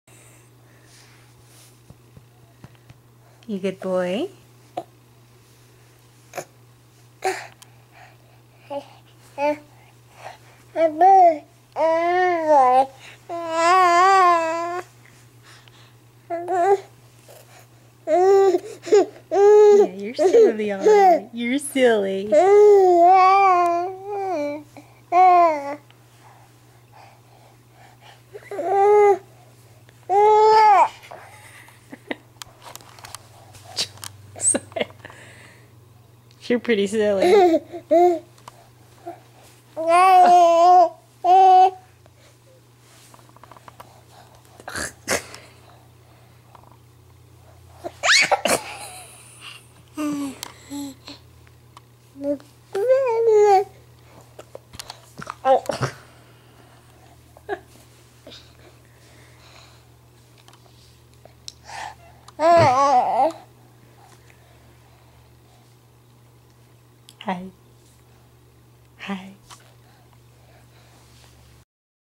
Snuggling with Mommy
Josiah has been in such a snuggly mood for the past week!